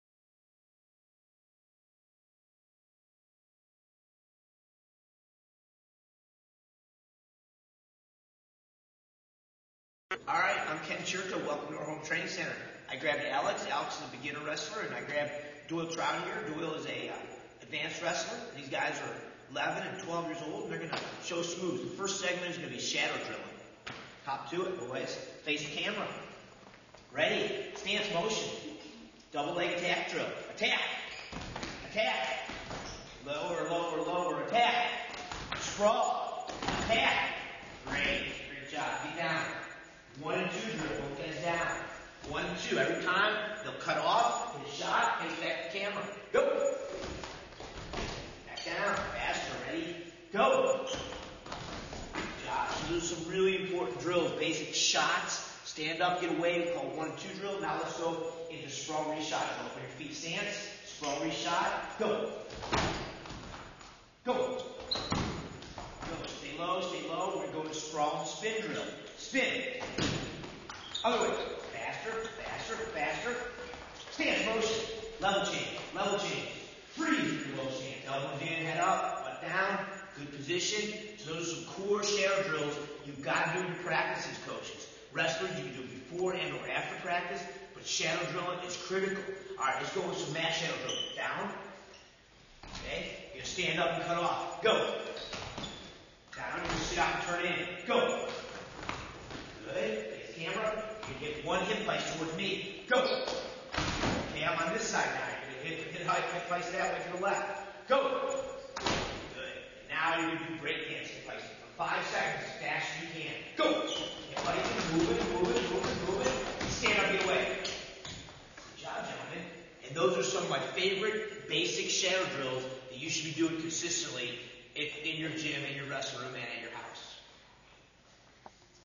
Ken Chertow Training - Shadow Drills
Ken works with Alex and Doyle to show the importance of shadow drilling.

Instructional DVDs, wrestling gear, and more available at wearandgear.com!